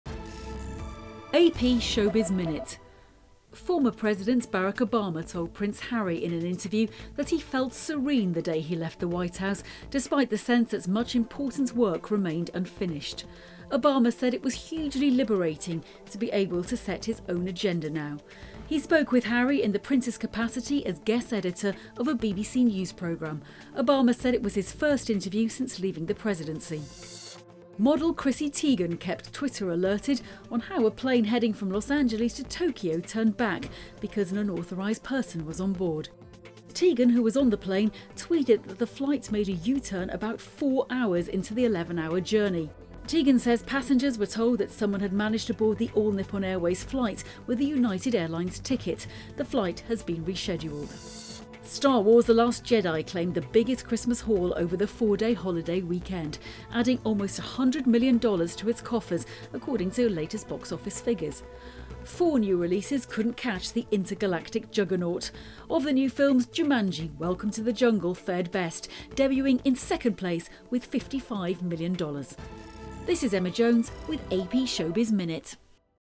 ShowBiz Minute: Obama, Prince Harry; Teigen; Box Office
Prince Harry interviews Barack Obama; Chrissy Teigen tweets about plane turn around; "Star Wars: The Last Jedi" tops holiday weekend box office. (Dec. 27)